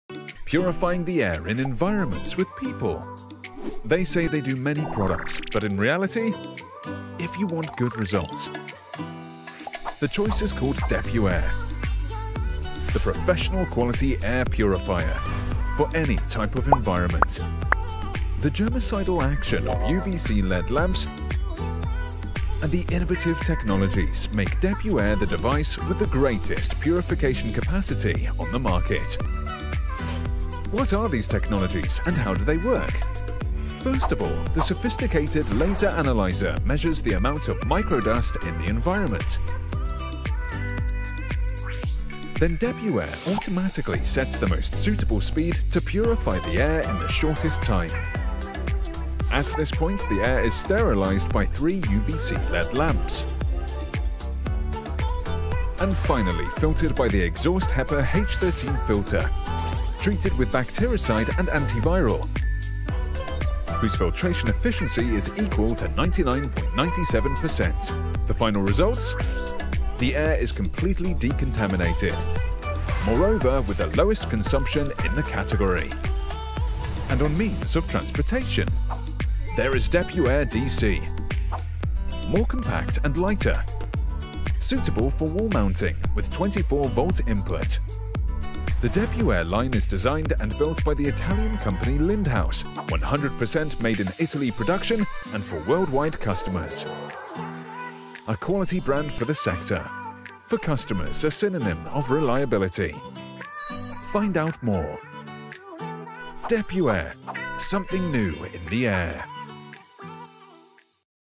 How does the Lindhaus Depur Air work
DepurAir professional air purifier
STRENGTHS

It changes 190 m³/h of air (= room of about 70 m2)
Quiet: 46/49 db(A) based on the speed
LCD display with programming:
POWER ON/OFF, AUTOMATIC, 4 SPEEDS: NIGHT, V2, V3, V4

Laser analyzer PM2,5 (Optional on Battery)
Automatic adjustment based on the quantity of detected micro dust.
Air released into the environment after passing through the hepa filter H13
100% stainless steel
100% made in Italy by Lindhaus

Electric version for wall or table

Designed to be used in environments with presence of people: clinics, shops, offices, car showrooms, hairdressers, wellness centers, Hotels, Restaurants, etc…

24VDC version

DepurAir 24VDC version has the same characteristics but in addition:

24V input to be powered in means of transport with on board people: buses, trains, airplanes